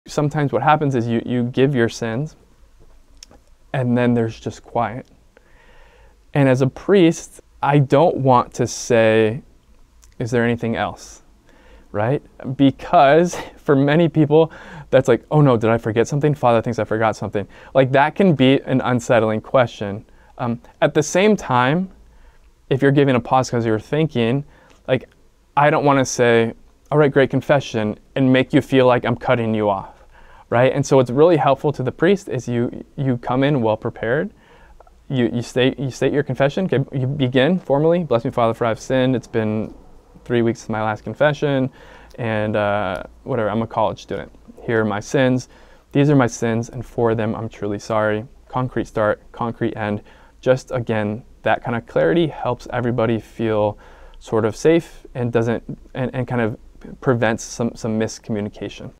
Making a Concrete End to a Confession
Fr. Mark-Mary provides us a good way to inform the priest that you have finished stating your sins during Confession.

—  SOCIAL MEDIA  —